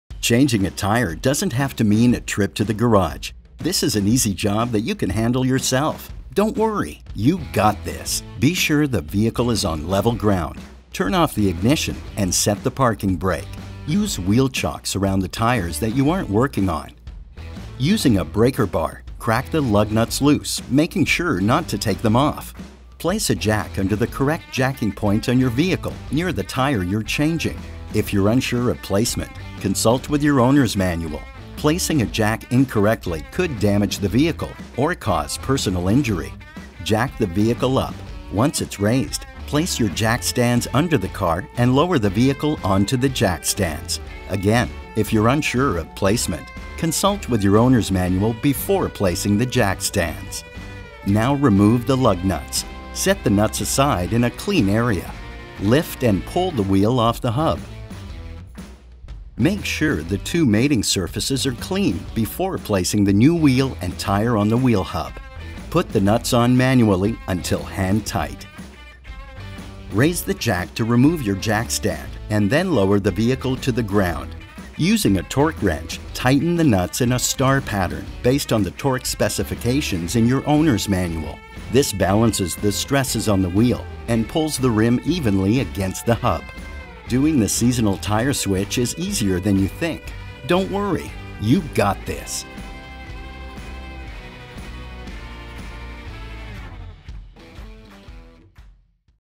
How to Change Your Tires
Learn how to do the seasonal tire switch.